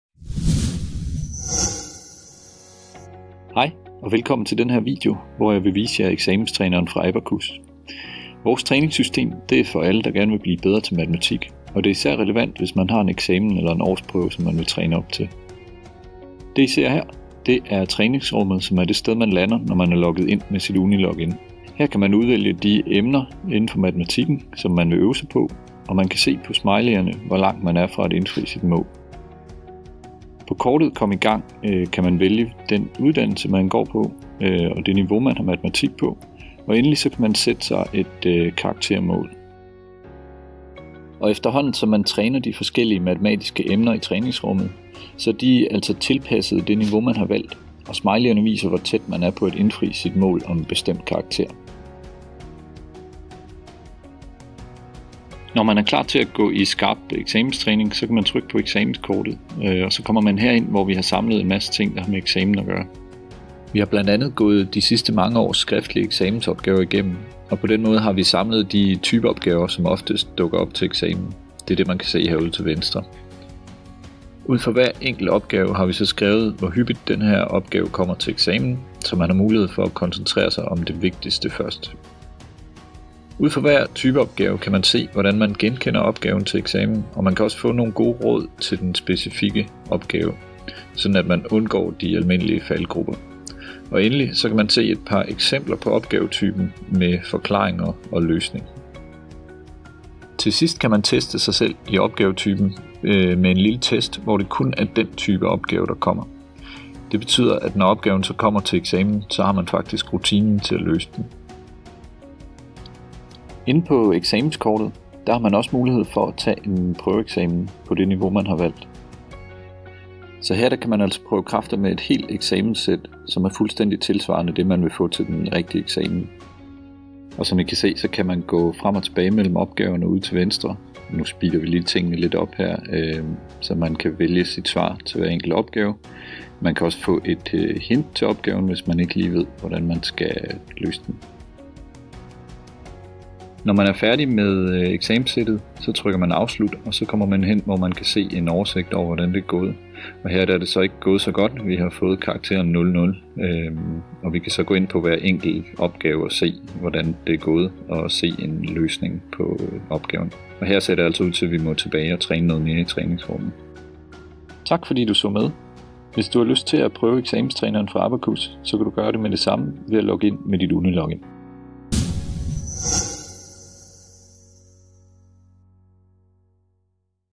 ABaCus eksamenstræner
Her kan du se en kort præsentation af eksamenstræneren fra ABaCus. Prøv selv på